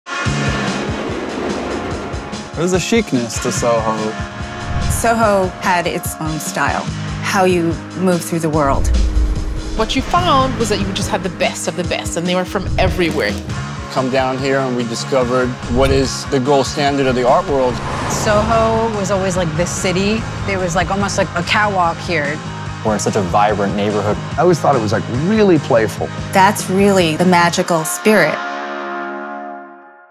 UNIQLO commercial "The Spirit Of Soho" Shot on ARRI ALEXA MINI LF + Leica R
Uniqlo Commercial featuring @futuradosmil Tabatha Mcgurr @ryanmcginleystudios @nicolavassellgallery @sohomemory @carlomccormickick + Tristan Hughes-Freeman's and the impact of soho on their lives and their work.

Directed by: @joebressler
DoP: @s.mastorelli
Steadi: @johnkopec
1st ac: Mike Ciecierski
2nd ac: @alex.huang_
Gaffer: @johnizarpate
Key grip: Jonathon Saavedra
Sound: @rjellenberg
Pa: @brandonstepanow
Edit: @lobedobe
Color: @jackcaswell @forager.tv
Sound mix: @baritobobb
Very special thank you: @newyorkcitycameracompany